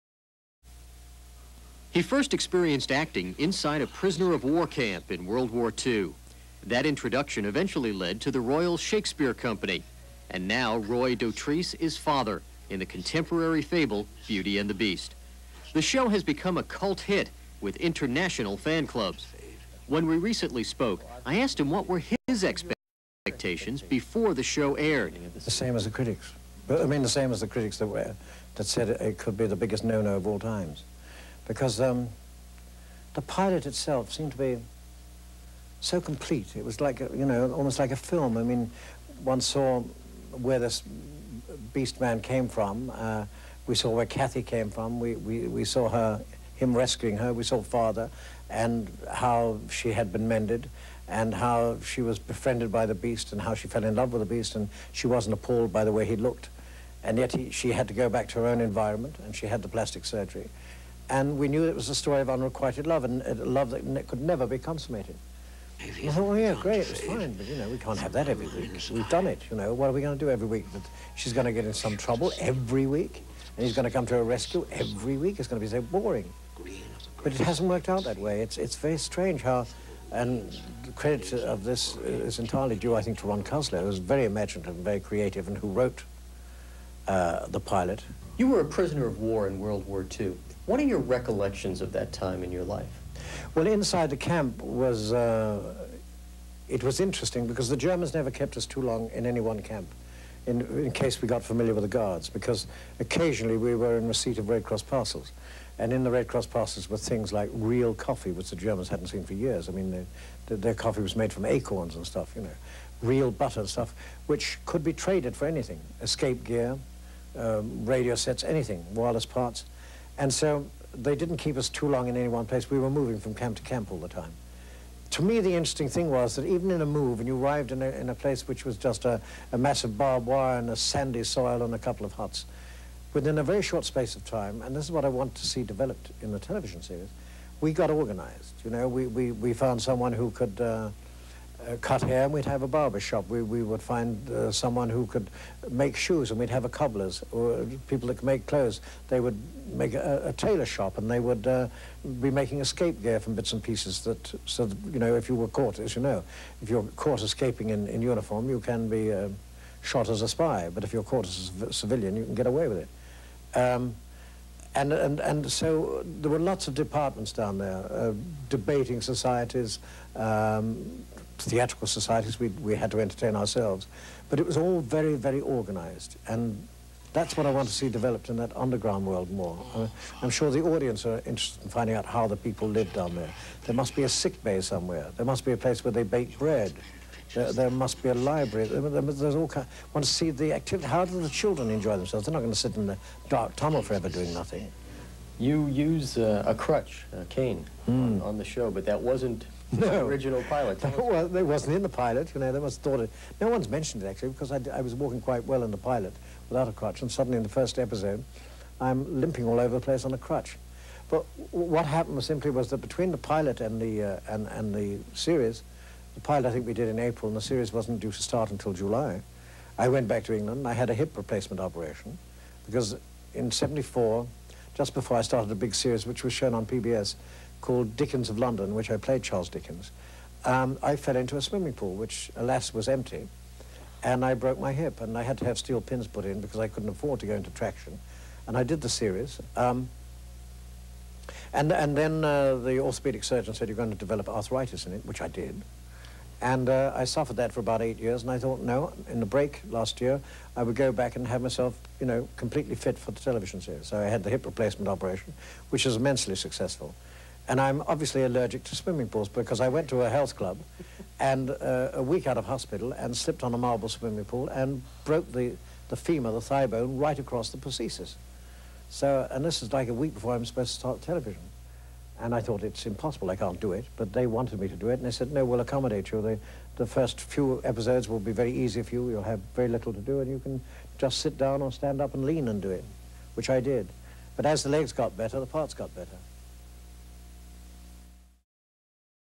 Roy Dotrice interview about Beauty and the Beast
Thanks to Je for saving this interview with Roy/Father. It is interesting why he was on a crutch. Do not go near swimming pools!

May the Tunnels always be with you...